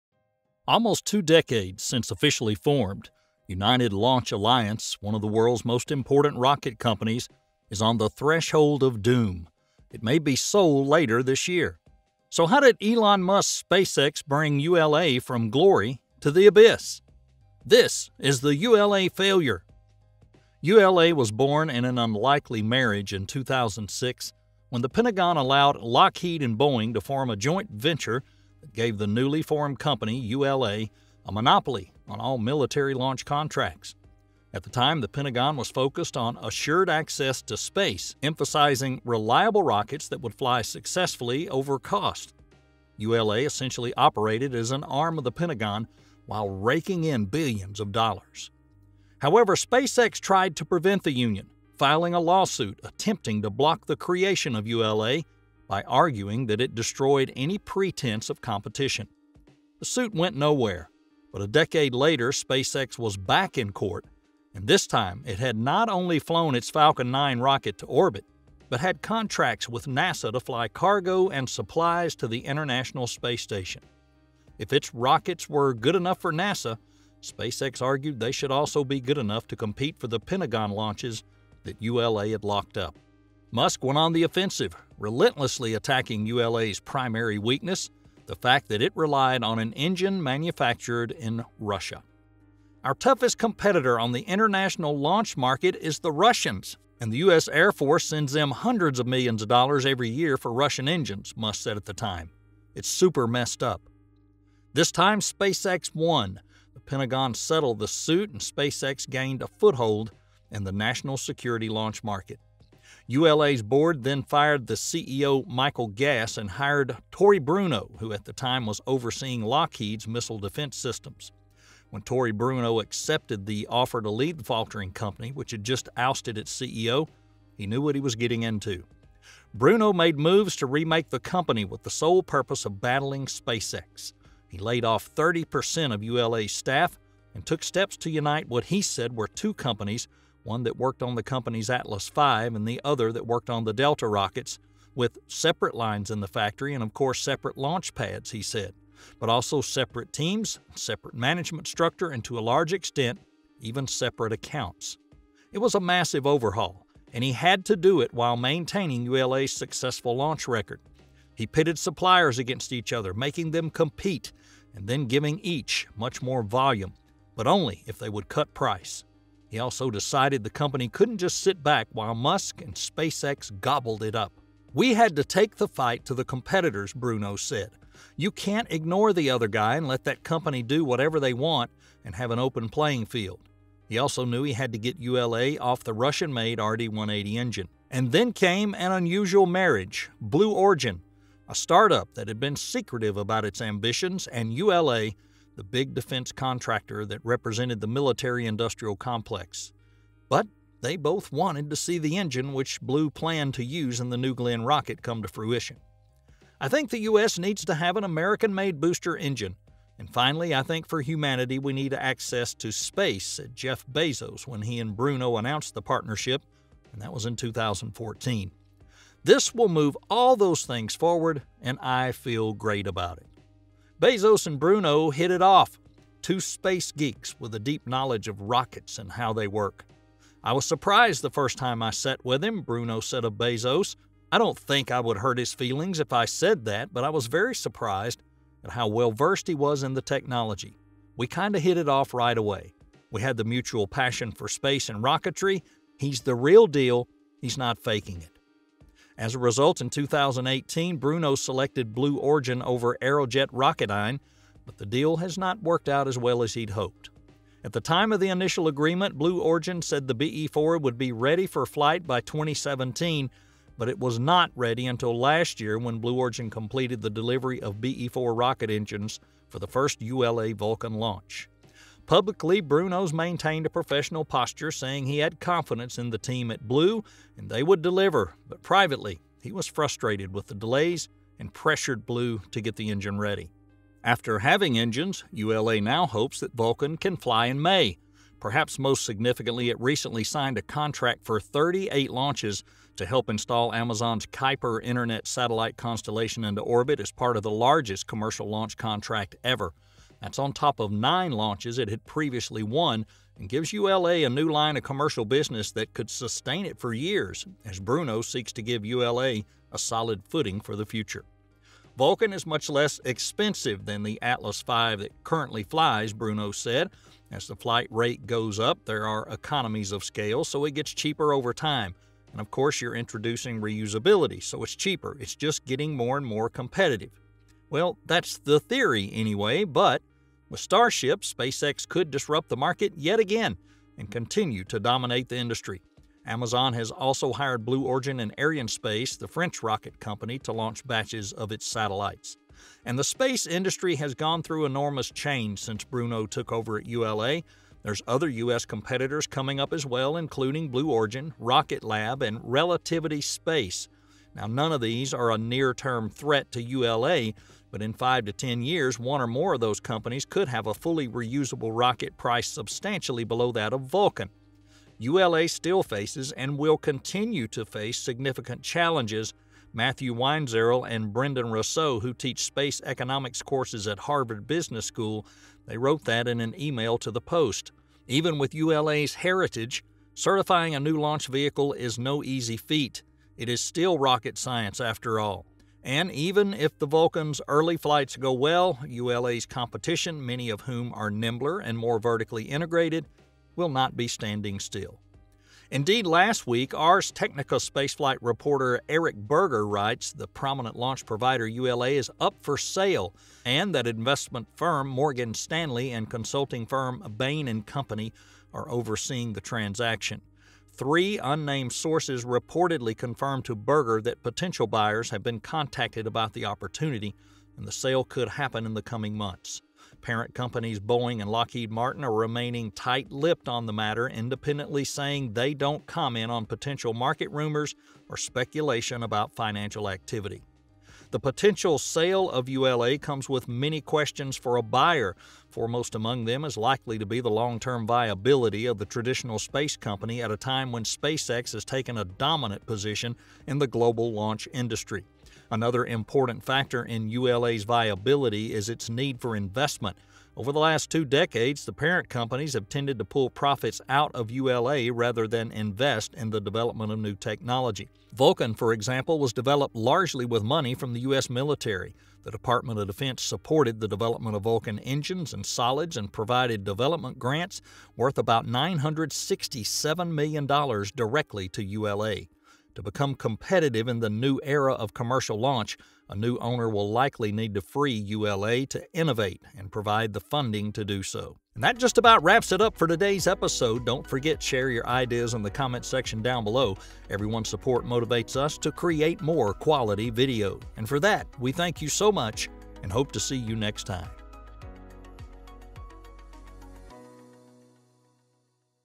Game over! ULA failure...
1, HUGE THANKS TO
Washington Post
Arstechnica

ULA failure!

Almost two decades since officially formed, United Launch Alliance, one of the world's most important rocket companies is on the threshold of doom. It may be sold later this year.
Well, how did Elon Musk's SpaceX bring ULA from glory to the abyss?
This is ULA failure!!!
ULA was born in an unlikely marriage in 2006 when the Pentagon allowed Lockheed and Boeing to form a joint venture that gave the newly formed company, ULA, a monopoly on all military launch contracts. At the time, the Pentagon was focused on “assured access to space,” emphasizing reliable rockets that would fly successfully, over cost. ULA essentially operated as an arm of the Pentagon, while raking in billions of dollars.
However, SpaceX tried to prevent the union, filing a lawsuit attempting to block the creation of ULA by arguing that it “destroyed any pretense of competition.”
That suit went nowhere. But a decade later, SpaceX was back in court, and this time it had not only flown its Falcon 9 rocket to orbit, but had contracts with NASA to fly cargo and supplies to the International Space Station. If its rockets were good enough for NASA, SpaceX argued, they should also be good enough to compete for the Pentagon launches that ULA had locked up.
Musk went on the offensive, relentlessly attacking ULA’s primary weakness — the fact that it relied on an engine manufactured in Russia.